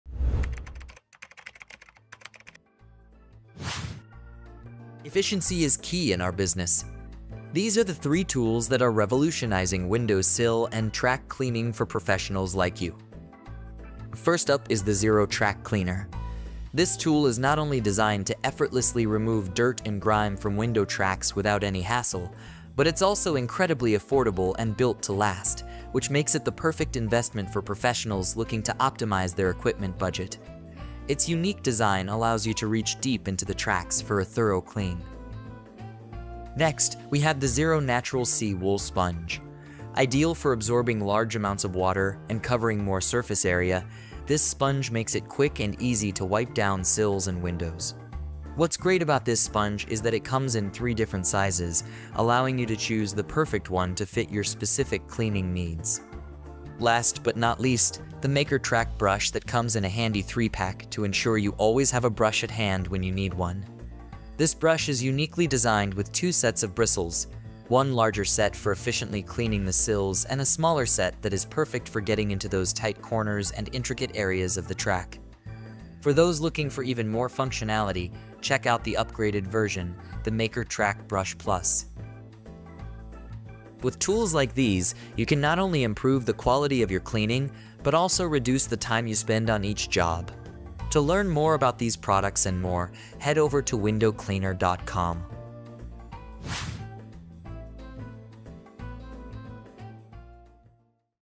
3 Must-Have Tools For Clean Window Tracks!
Say goodbye to stubborn dirt in window tracks and sills with our pick of the top three tools for cleaning tracks & sills effortlessly. Simplify your cleaning process and save time!